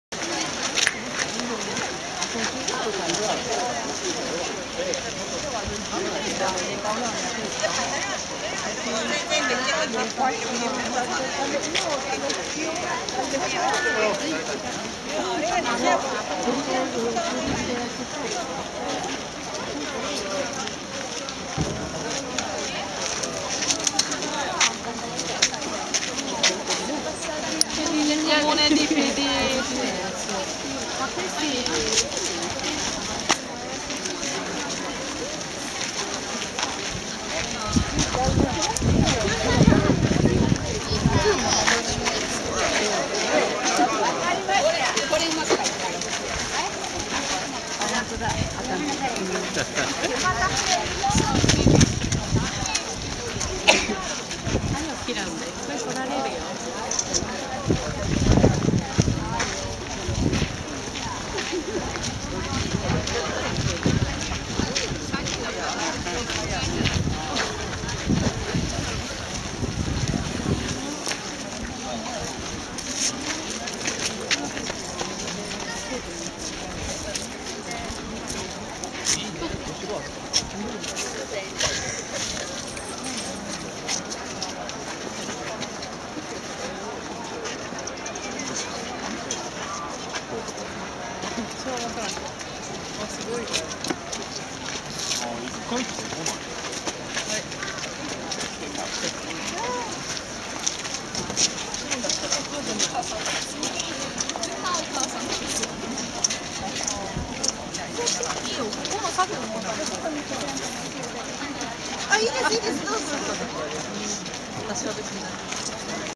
Il castello di Himeji marzo 2015 - Himeji castle
Finita la visita alla torre principale del castello qualche minuto di sosta nel piazzale sottostante per consentire al gruppo di radunarsi.
Una giornata spettacolare per un posto favoloso!